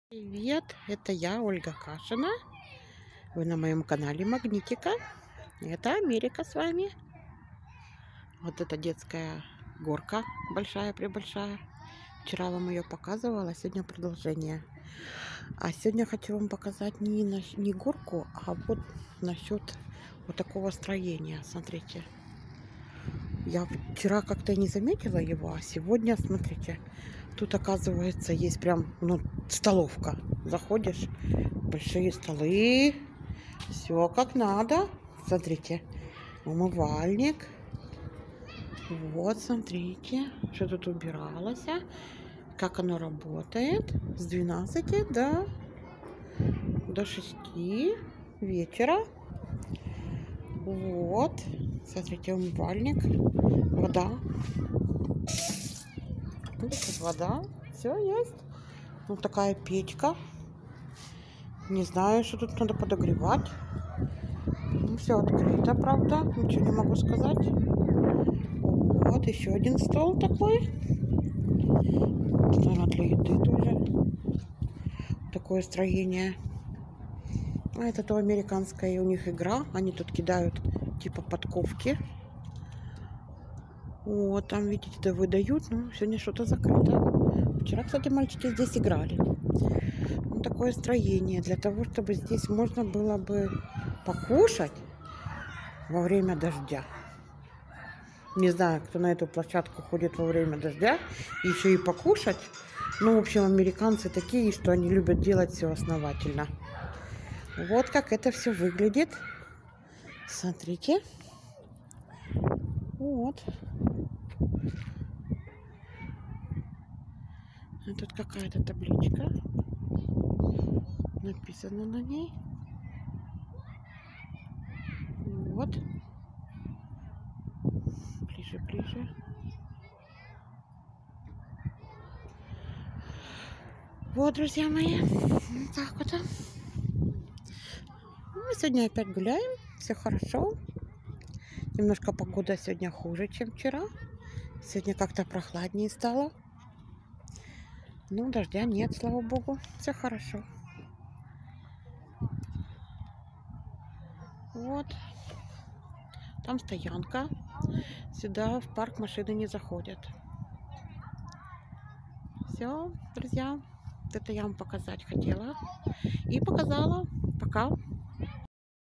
Таких удобств у нас точно нет.🇺🇲
Все про парк. Олимпия штат Вашингтон США.